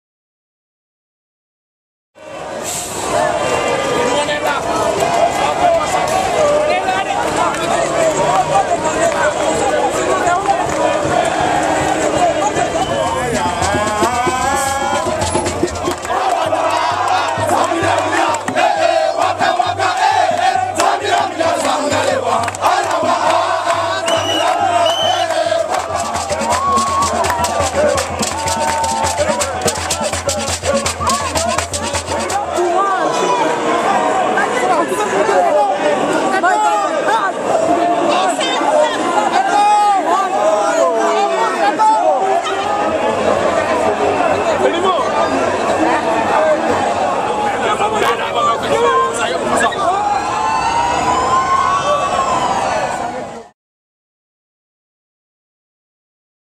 Indomitable Lions Arrive Dakar 24 March 2011
Film de l'arrivée des Lions Indomptable à Dakar le 24 Mars 2011 pour le match contre le Sénegal prévu le 26 Mars. On y voit l'arrivée des supporters camerounais basé au Sénégal, qui vont animé l'aéroport International de Dakar avec danse et tam-tam jusqu'a l'arrivée de Samuel Eto'o et ses collègues. Puis le car transportant les Lions s'ebranle pour l'hotel Radisson Blu. Created on March 25, 2011.